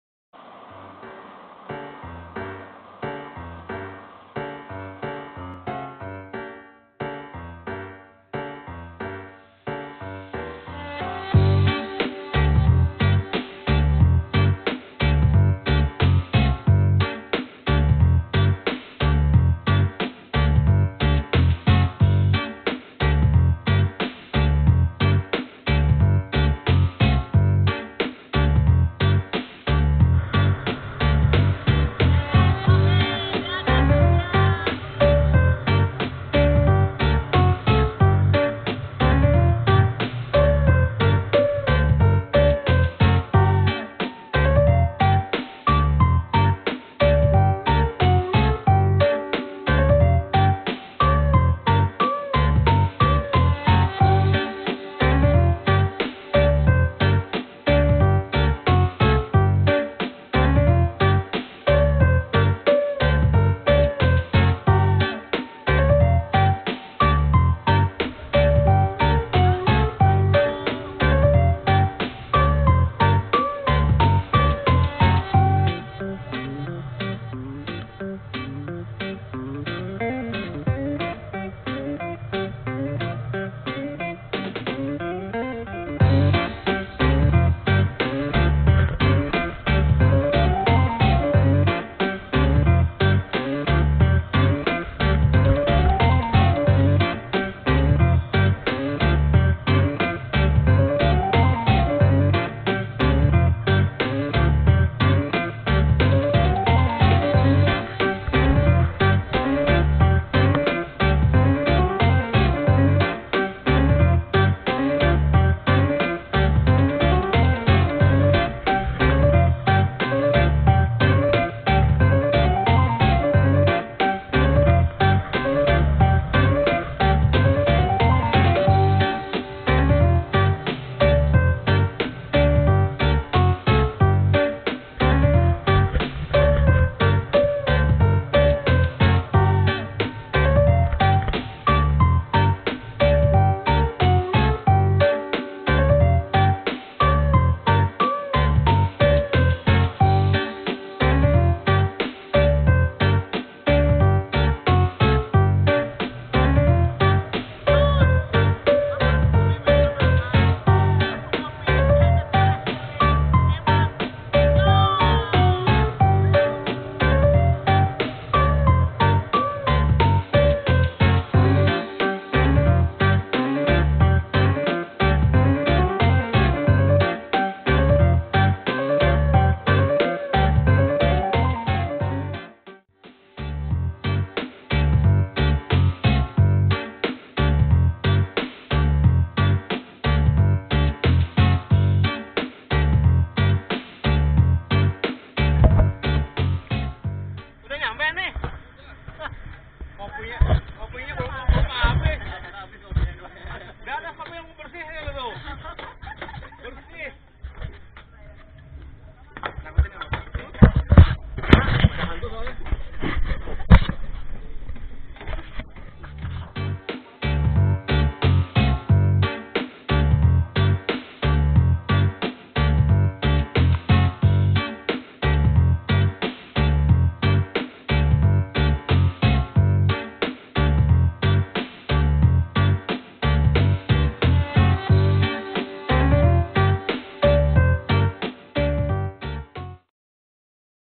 Ngopi sambil ber Parasailing
acara adventure kali ini adalah Minum Kopi sambil Parasailing dikawasan Tanjung Benoa Bali, saat Solo Riding Jakarta - Bali dgn mengendarai sepeda motor Kawasaki Versys.

Minum kopi di Udara sambil ber Parasailing sensasi nya memang luar biasa, view landscap sejauh mata memandang, melihat garis bumi yg bulat serta hembusan angin yg sejuk.

sensasinya luaar biasaa...

salam adventure